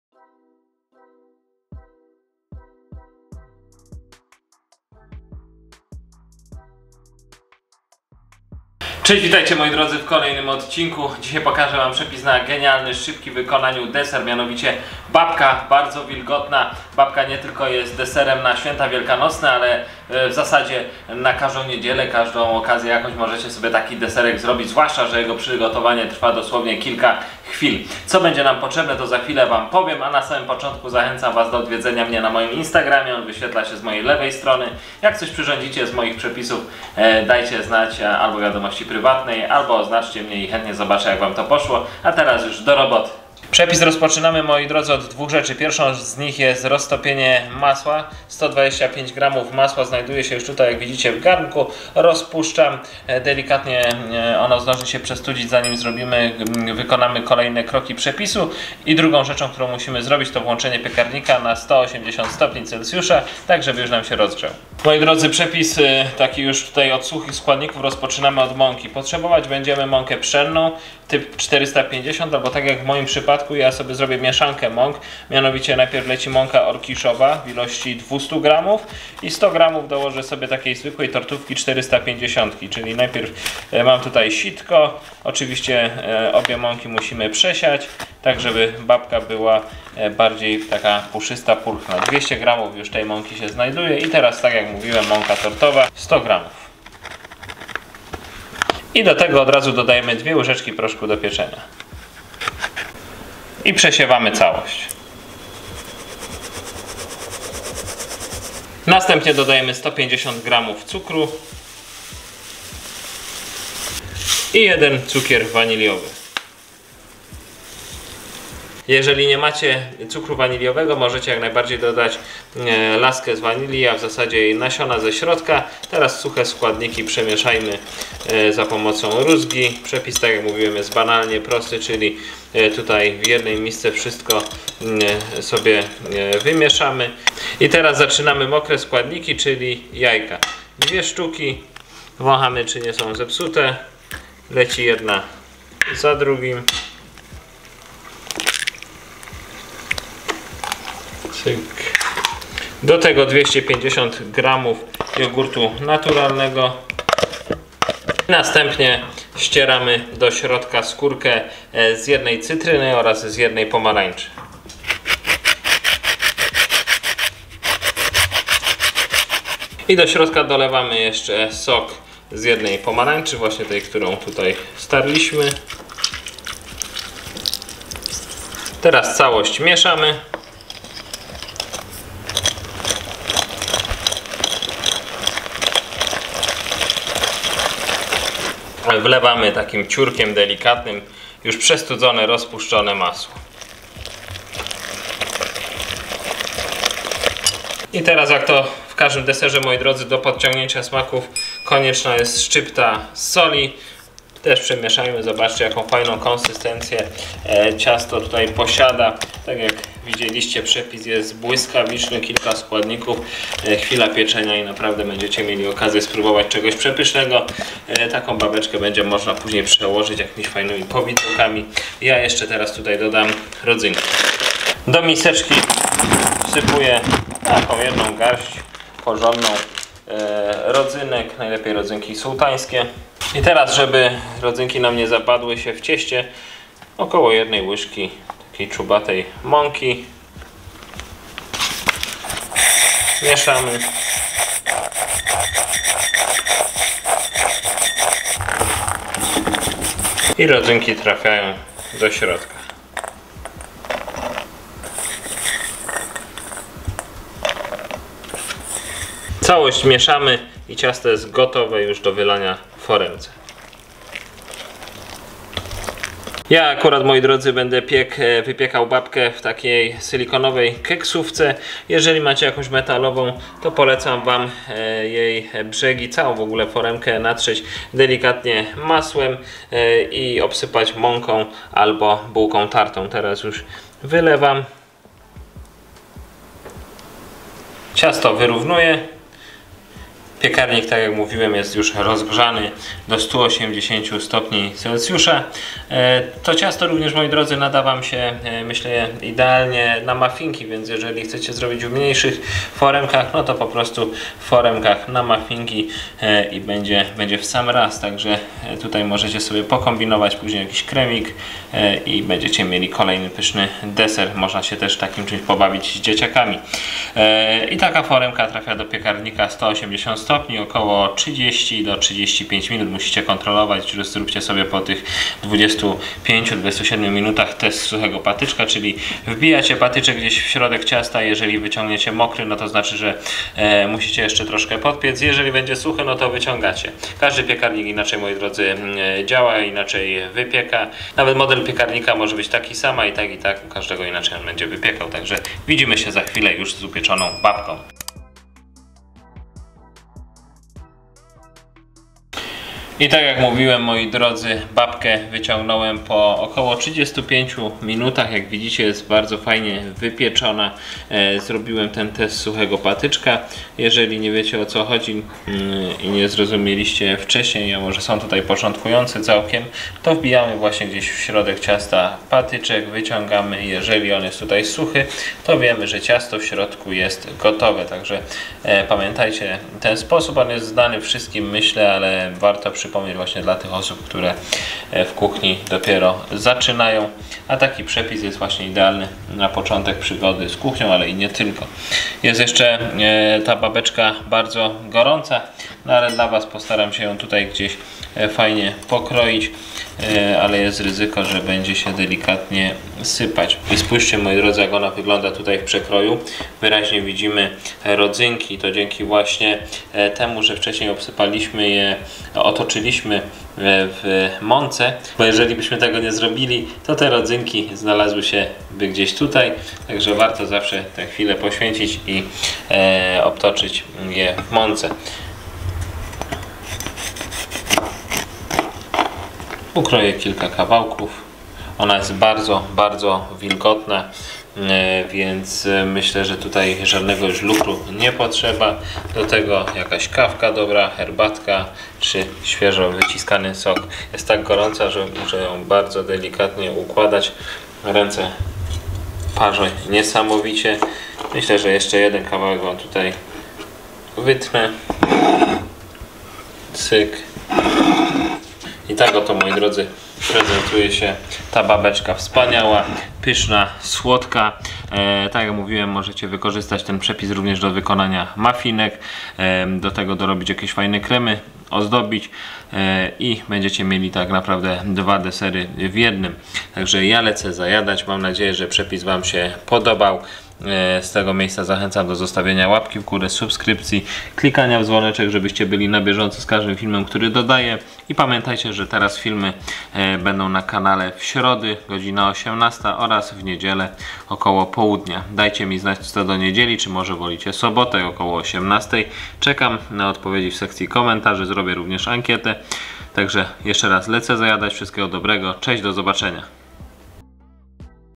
TAKIEGO PUSZYSTEGO I WILGOTNEGO CIASTA NIE JEDLIŚCIE! CIASTO W 10 MINUT!
Babka to nie tylko ciasto na Wielkanoc czy od święta. Ten przepis jest szybki i tani w wykonaniu, a dzięki mieszance mąk ciasto jest bardzo wilgotne. Koniecznie wypróbuj ten przepis.